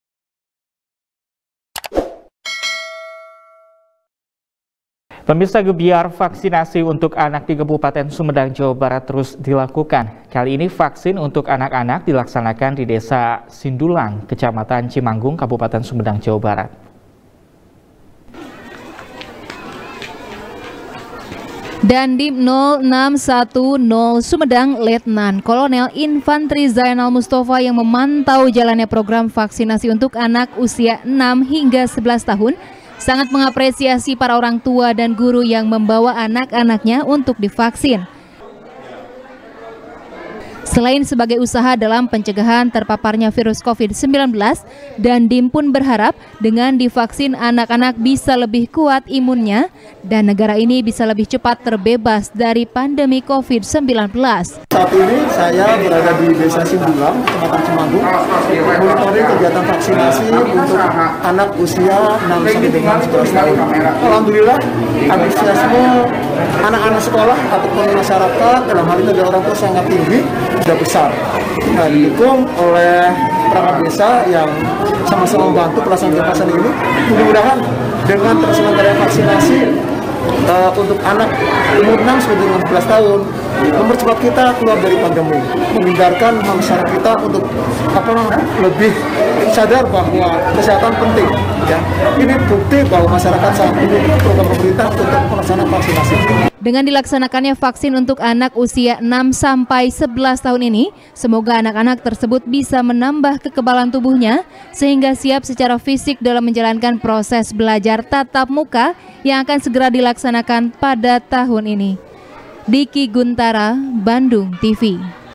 Gebyar Vaksin Anak Gencar Dilaksanakan | SBR | BANDUNGTV
SBR | BANDUNGTV | IRFAN RAMANDA | Gebyar Vaksinasi Untuk Anak Di Kabupaten Sumedang, Jawa Barat, Terus Di Lakukan. Kali Ini, Vaksin Untuk Anak-Anak, Dilaksanakan Di Desa Sindulang, Kecamatan Cimanggung, Kabupaten Sumedang, Jawa Barat.